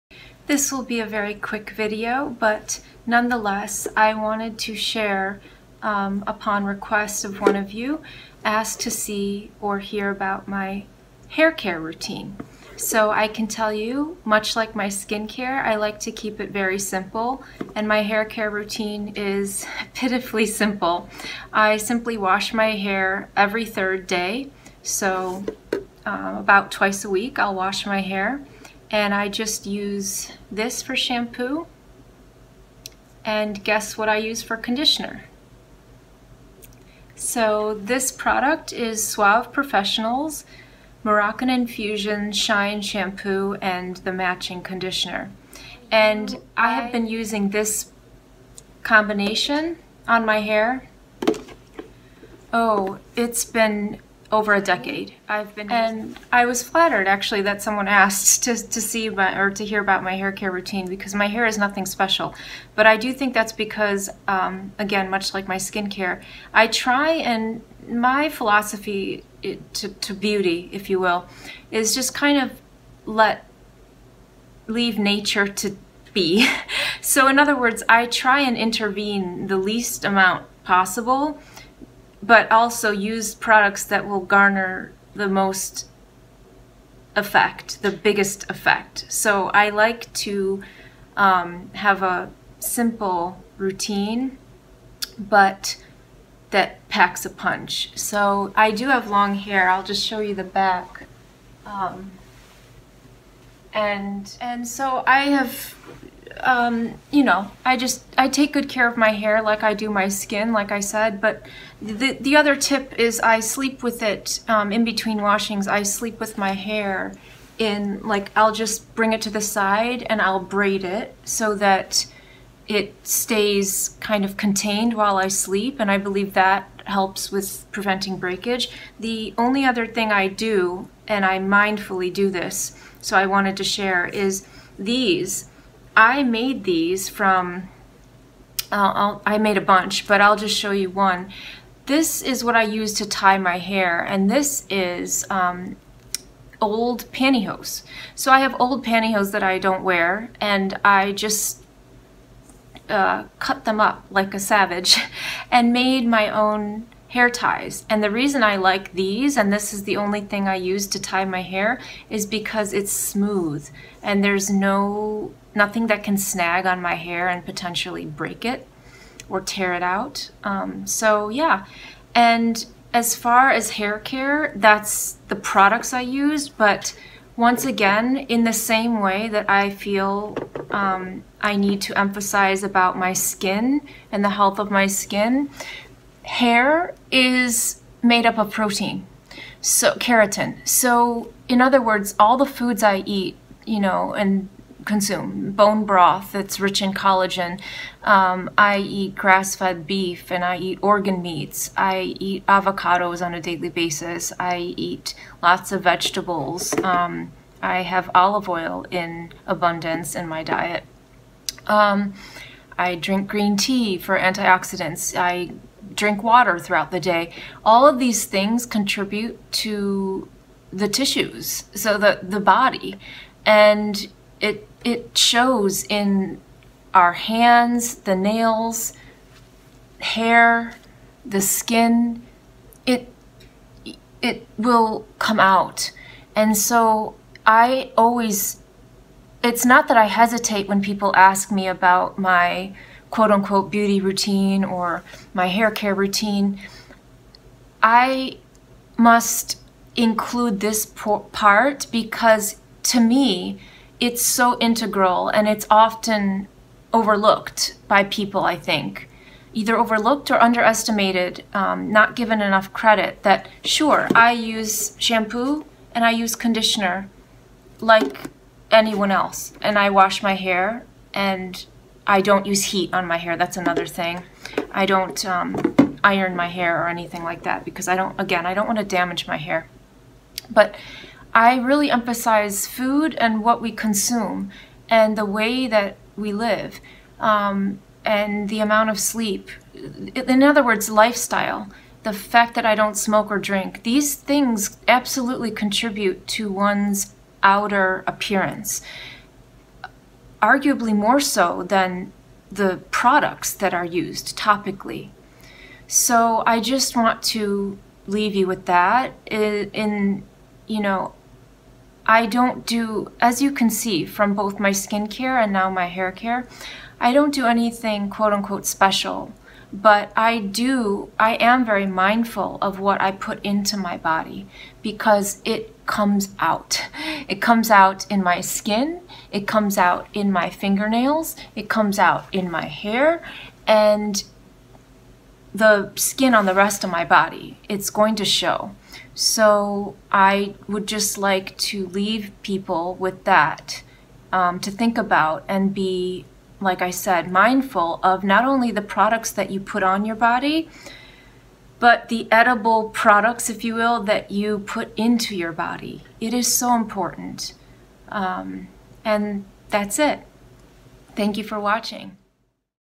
My Hair: How I Keep it Long & Healthy + Products, Habits and What I Eat (Yes)
At the risk of beating a proverbial dead horse, the first thing I think of whenever anyone asks about my hair or skin care routine isn't what I use in the way of products--it's my lifestyle habits and the foods I consume. Why? Because, the foods we consume dictate the growth, integrity, maintenance and appearance of our bodily tissues! YOU ARE WHAT YOU EAT. We've all heard it before, and it's the truth. Healthy skin, healthy hair--it comes from WITHIN first and foremost.

Shampoo & Conditioner I use

Skincare I use on my face + body:
Facial Cleanser (AM & PM)
Moisturizer (AM & PM)
Micellar Water
Organic Castor Oil (used in place of eye cream)
Sunscreen (Facial AM Daily)
Neutrogena Healthy Defense SPF 50
Ceramide Healing Protectant
Petroleum Jelly (daily lip balm)
Neutrogena Moisturizer
Aquaphor (overnight lip treatment)
Alpha-hydroxy Acid Exfoliating Lotion
Cetaphil Daily Advance w/Shea Butter
Mineral Deodorant
Neutrogena Hydro Boost Body Gel Cream
Neutrogena Baby Mineral Sunscreen Stick SPF 60

Everyday items I use:
Exfoliating Body Care Gloves
My driving gloves
Sun protection sleeves (for driving)
My Yoga Mat

xo